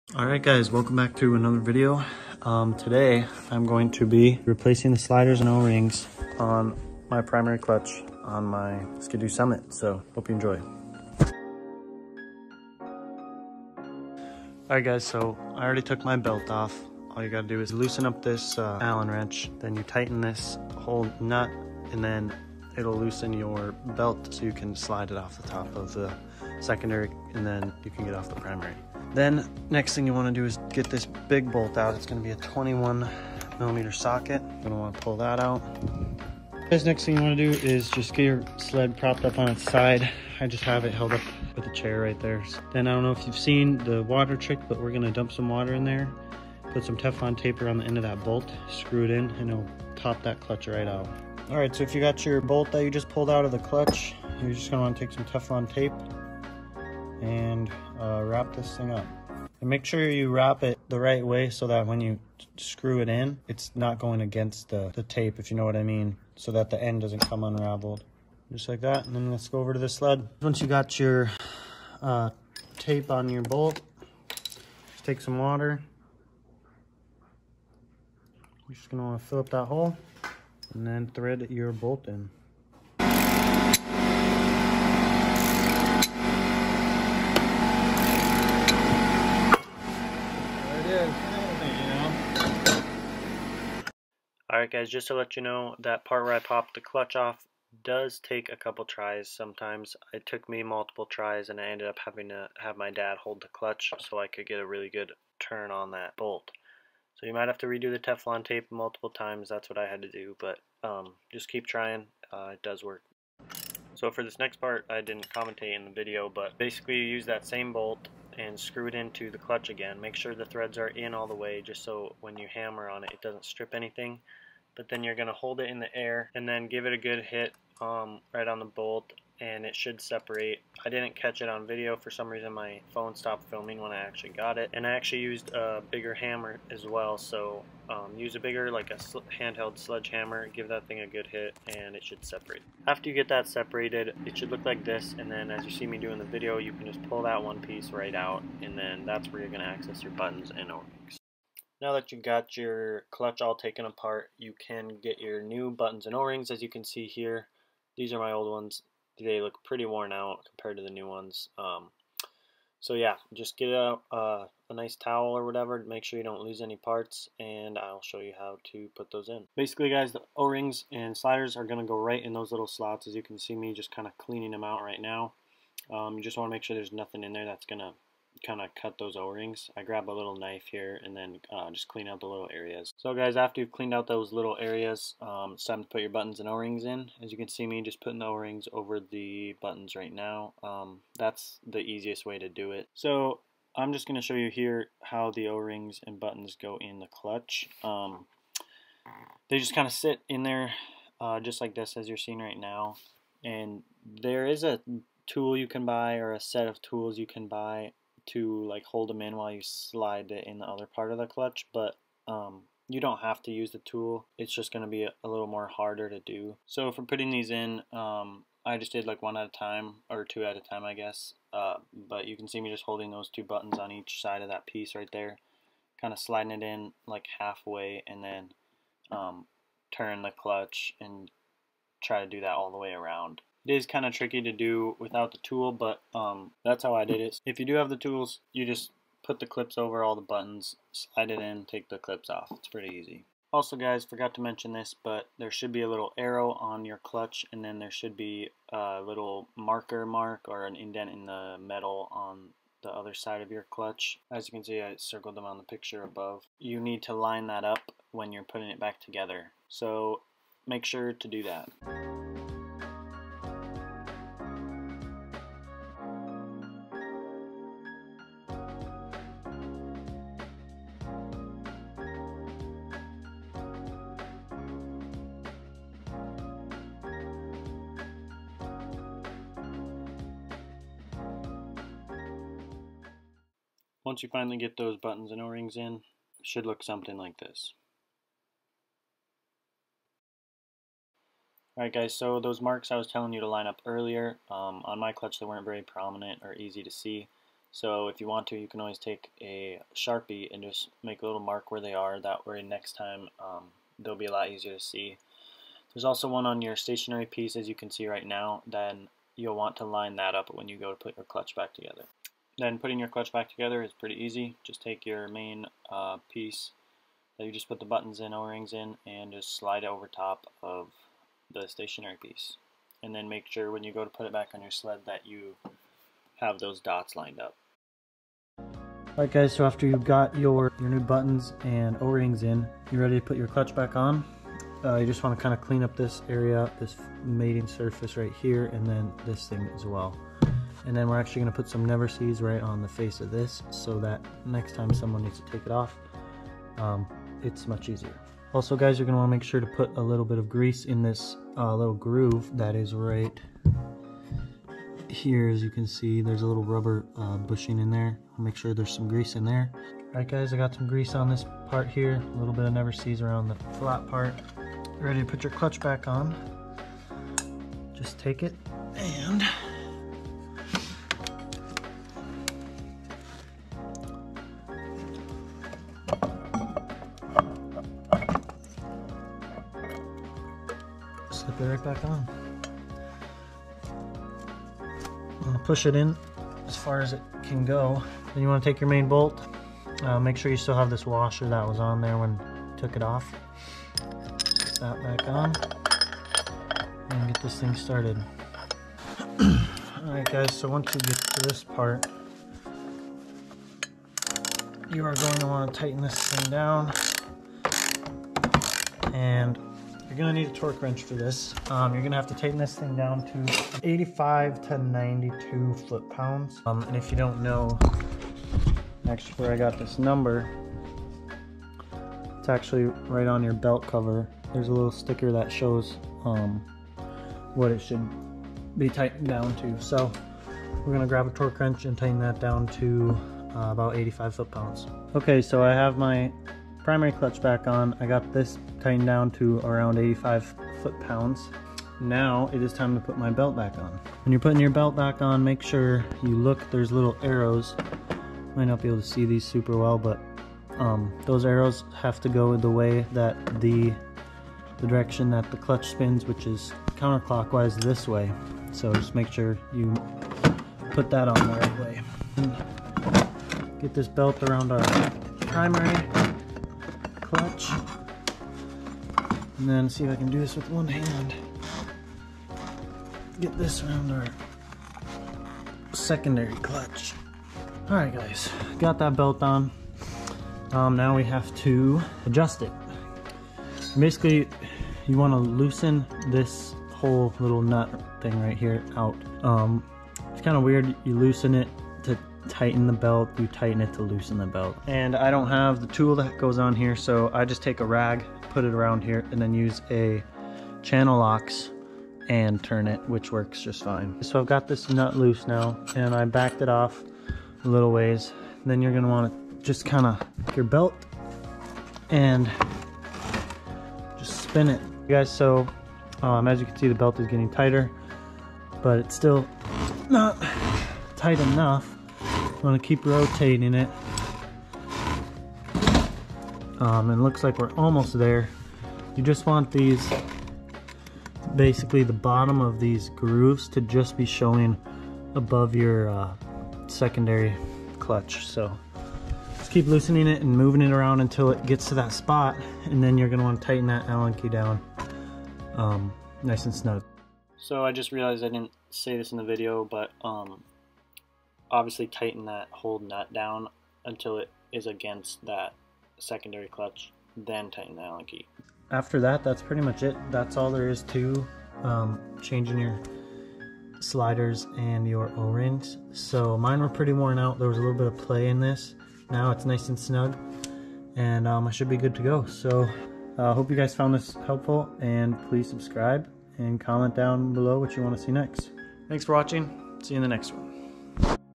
How to Replace Sliders and o-rings in primary clutch on a Ski-doo.
In this video I show you how to replace sliders and o-rings on s ski-doo snowmobile. I hope you found this helpful!